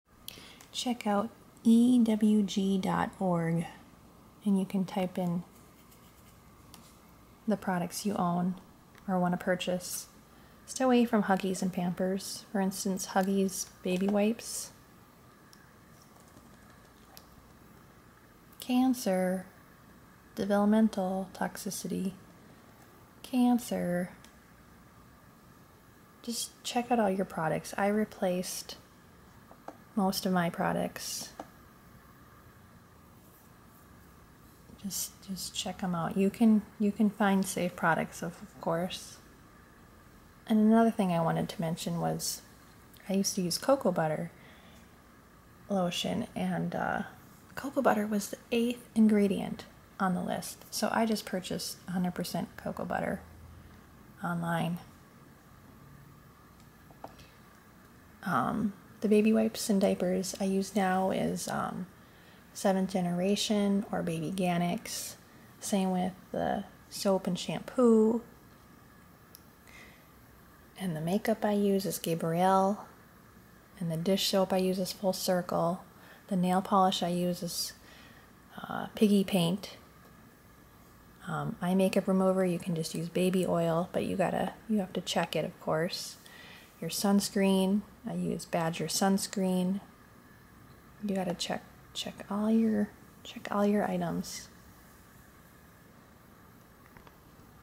Non toxic products. EWG.org
Stay away from Bath and Body works/Huggies/Pampers/Dawn/cover girl & maybelline makeup.